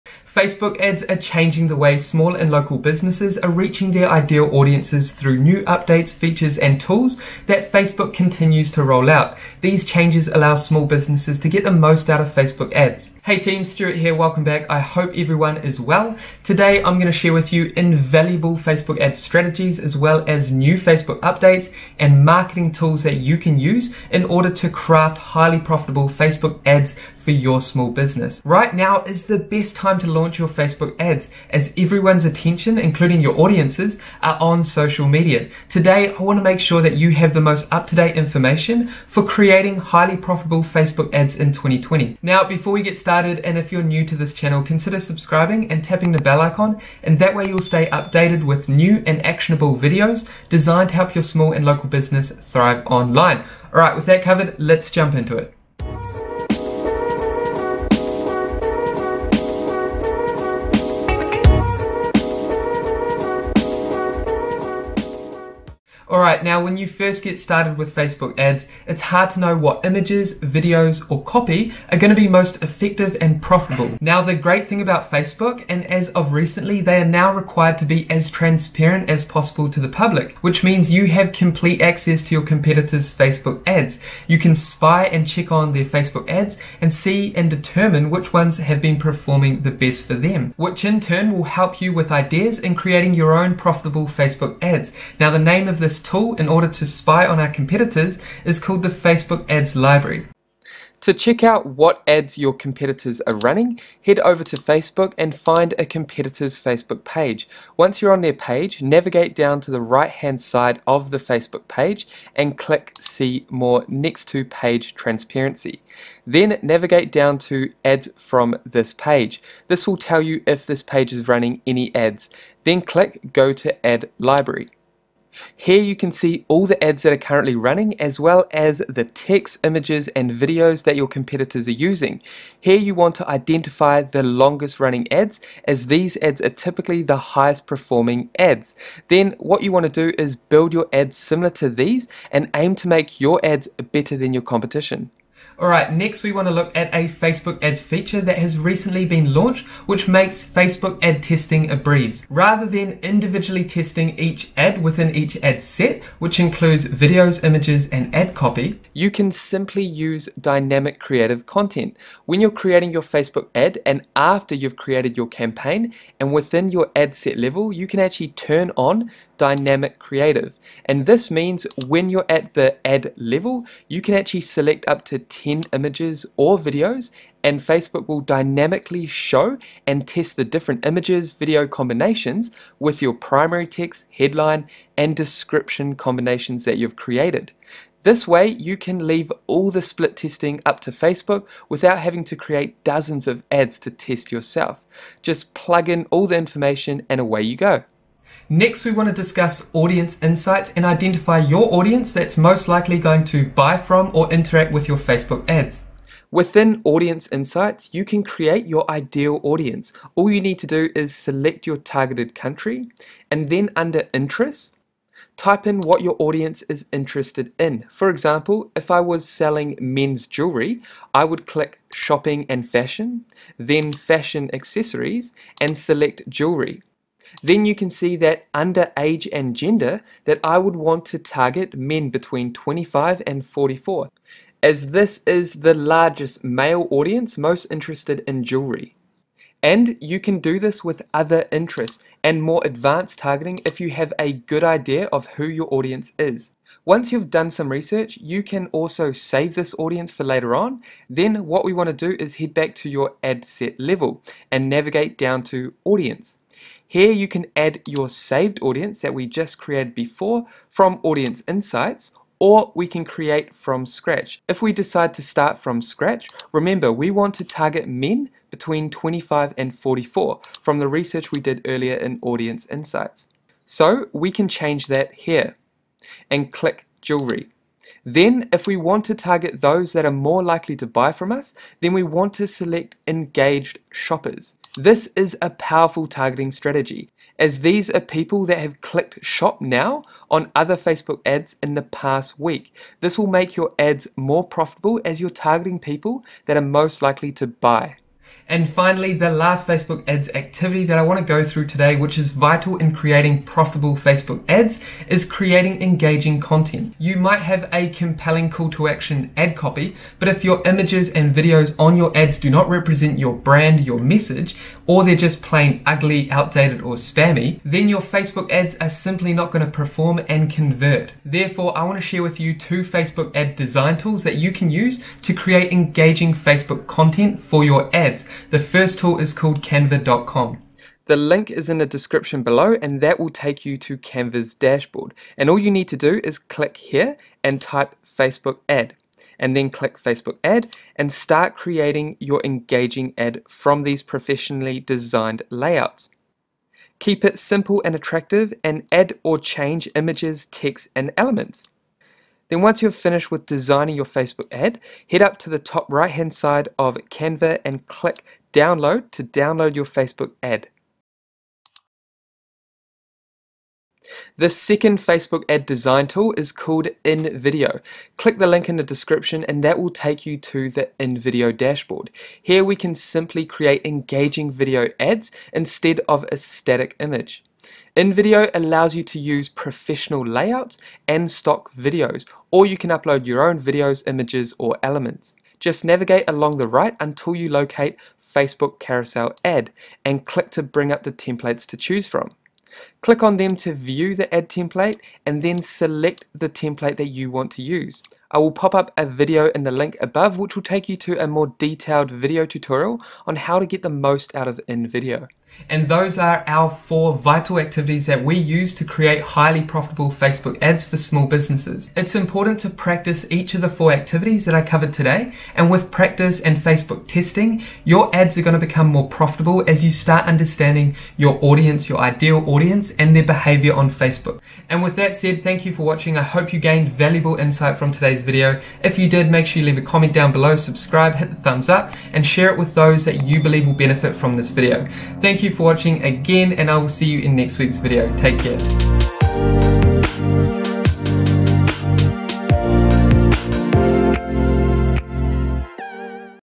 Facebook Ads Tutorial | Small Business Secrets to Creating Highly Profitable Facebook Ads
Today we discuss Facebook Ads in 2020 and Small Business secrets to creating highly profitable Facebook Ads.

Facebook Ads are changing the way small businesses are reaching their audiences through new updates, features and tools. The goal of this video is to ensure you have the most up to date information in order to create highly profitable Facebook Ads.

In this Facebook ads tutorial, we guide you through our effective strategies and tools that small business can use to take advantage and get the most out of Facebook ads in 2020.

Facebook ads for small business should be an essential part of their marketing strategies, especially during this global crisis as everyone's attention is on social media.

► Guide to creating profitable Facebook Ads in 2020: tutorial guide (four essential activities):

1. Facebook Ads Library 01:10
2. Dynamic Creative 02:29
3. Audience Insights & Engaged Audiences 03:21
4. Creating Engaging Content 05:05

Let me know if you found this Facebook Ads tutorial helpful.